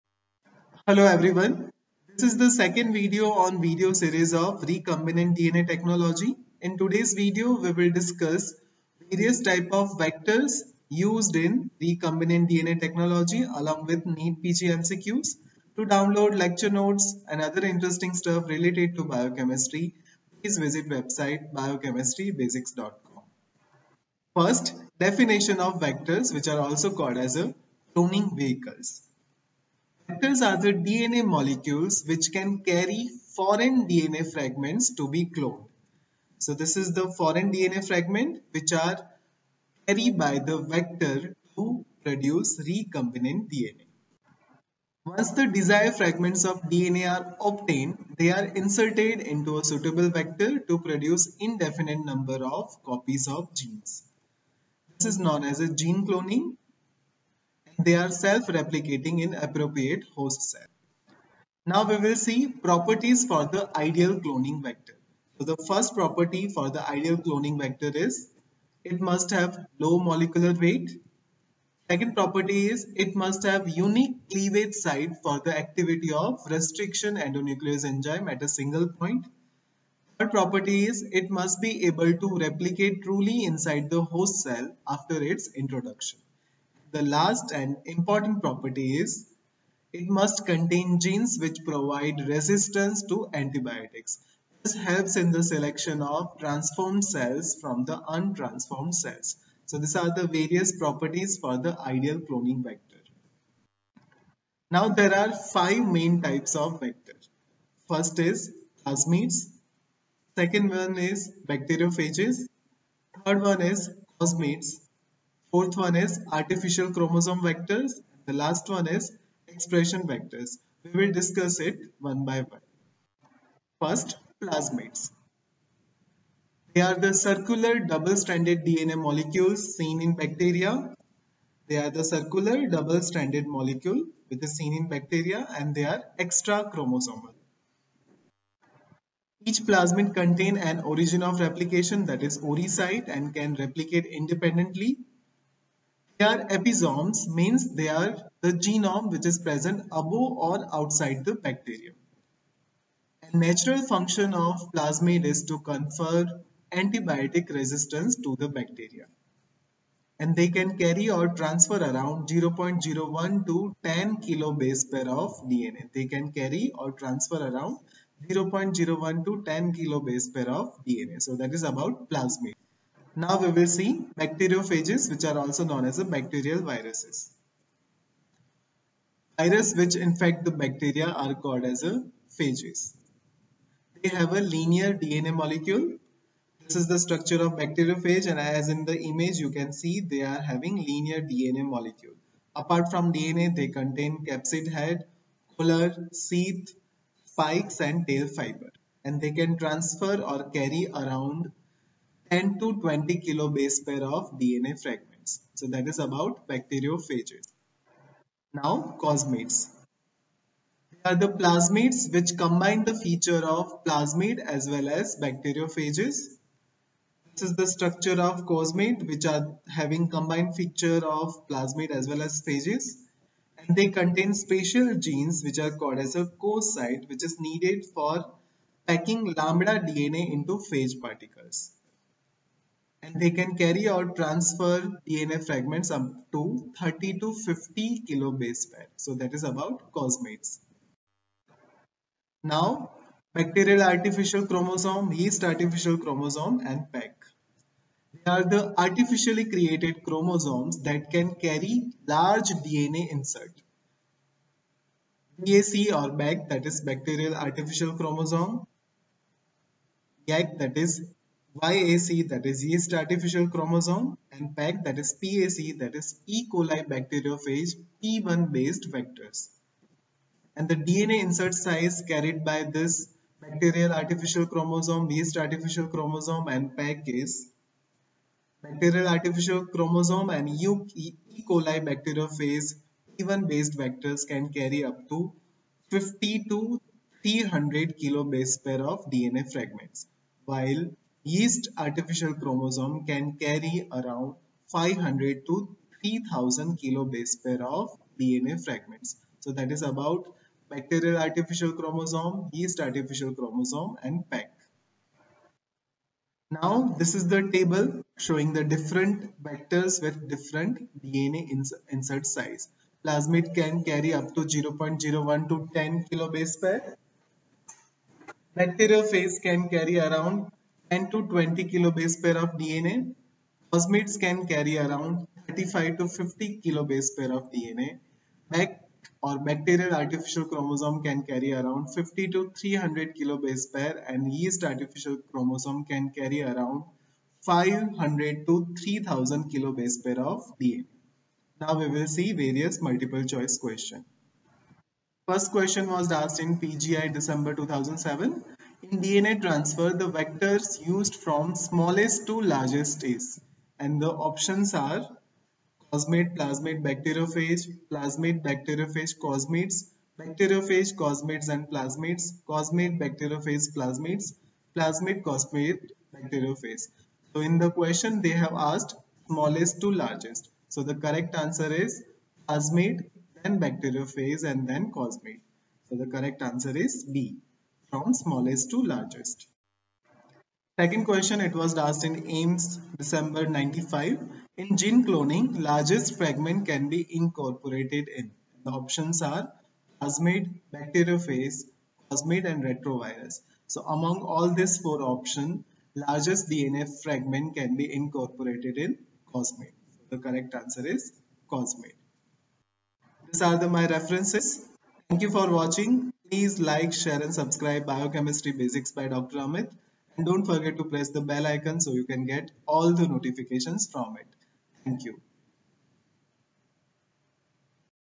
Types of Vectors Used In Recombinant DNA Technology || Cloning Vectors
Types of Vectors Used In Recombinant Dna Technology - This is the video on Types of Vectors (The Cloning Vehicle) Used In Recombinant DNA Technology with NEET PG MCQs
It contains
Definition of Vectors - 00:24 minutes
Properties for the ideal cloning vectors -  01:02 minutes
Types of DNA vectors (cloning vectors) - 01:48 minutes
Plasmids - 02:04 minutes
Bacteriophages - 02:55 minutes
Cosmids - 03:30 minutes
BAC PAC and YAC - 04:02 minutes
Comparison of DNA Vectors - 05:08 minutes
NEET PG MCQs - 05:47 minutes

                                       Vectors – The Cloning Vehicles
Vectors are the DNA molecules, which can carry foreign DNA fragment to be cloned

Once the desired fragments of DNA are obtained, they are inserted into a suitable vector (carrier molecule) to produce indefinite number of copies of genes.
This is known as gene cloning.
They are self- replicating in an appropriate host cell.

Vectors are of the following types
1) Plasmids
2) Bacteriophages
3) Cosmids
4) Artificial chromosome vectors
5) Expression vectors